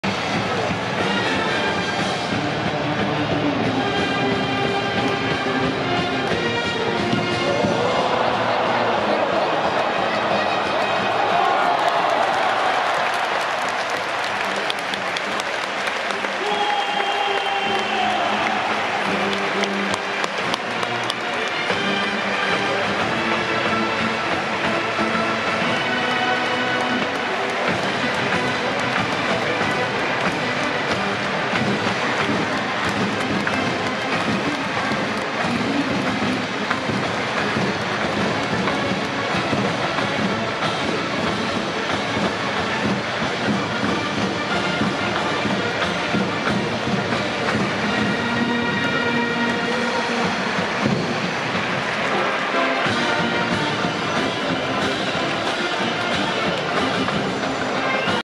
4番 内海優太《 ZOZOマリン右中間に木製バットでホームランです❗ 》侍ジャパンU-18壮行試合 高校日本代表 - 大学日本代表 2022年8月31日(水)
打った瞬間のタイミング、インパクト決まった感じ😊
確信ムランだと思われます